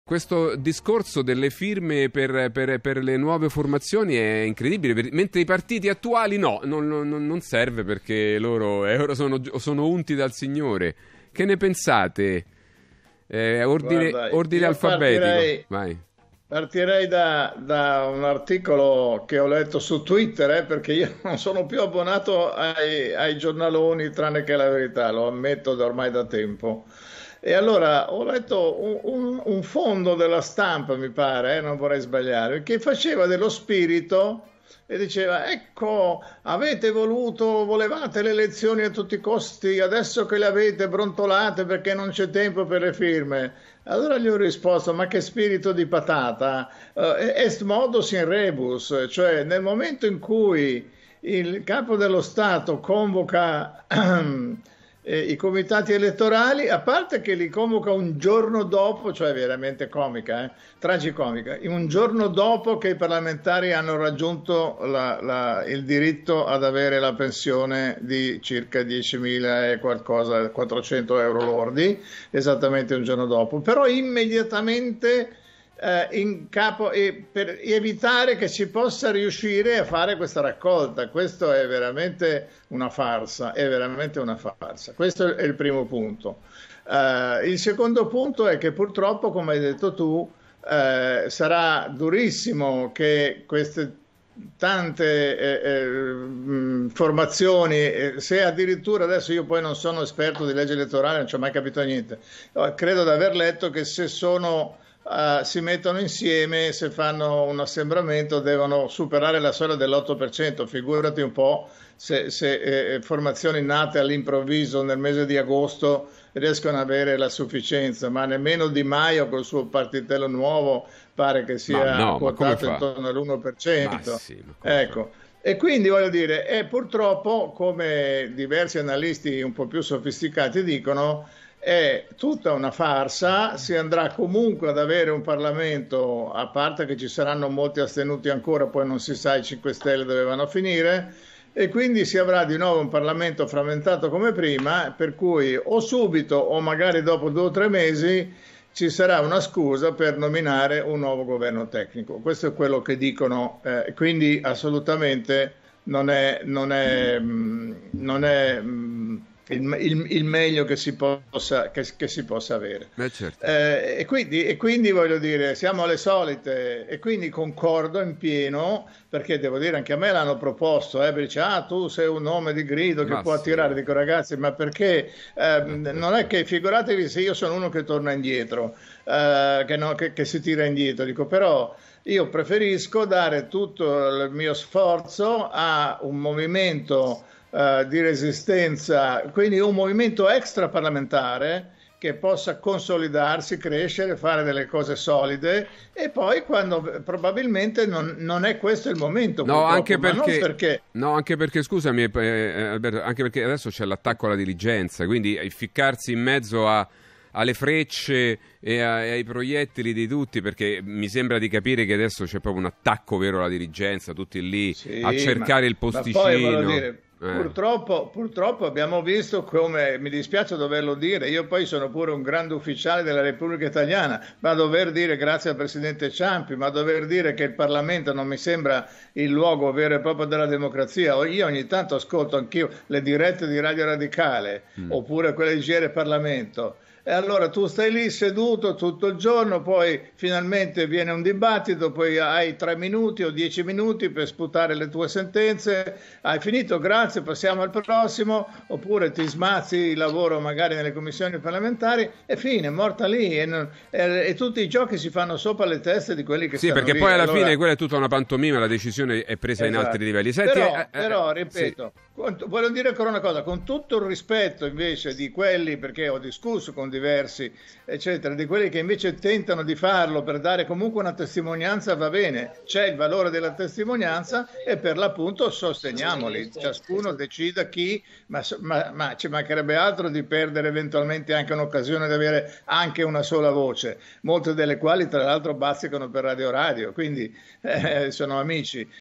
DEMOCRAZIA ALLO SBANDO ▷ PROF CONTRI: "RACCOLTA FIRME? UNA FARSA, SI TORNERÀ A UN GOVERNO TECNICO"
La campagnaelettorale in vista delle prossime elezioni di settembre è iniziata e già è evidente che per i piccoli partiti sarà molto difficile riuscire a raccogliere il numero di firme necessario per entrare in Parlamento. Non mancano le proteste di chi vede in questa misura un modo per limitare la democrazia e, soprattutto, il pluralismo, che della democrazia è corollario fondamentale. Tra questi, il Prof AlbertoContri, Docente di Comunicazione Sociale.

“Purtroppo, come diversi analisti hanno osservato, è tutta una farsa, si andrà comunque ad avere un Parlamento frammentato per cui subito, o magari dopo due-tre mesi, ci sarà una scusa per nominare un nuovo governo tecnico. Io preferisco dare tutto il mio sforzo a un movimento extraparlamentare che possa consolidarsi e crescere. Il Parlamento non mi sembra il luogo vero e proprio della democrazia“.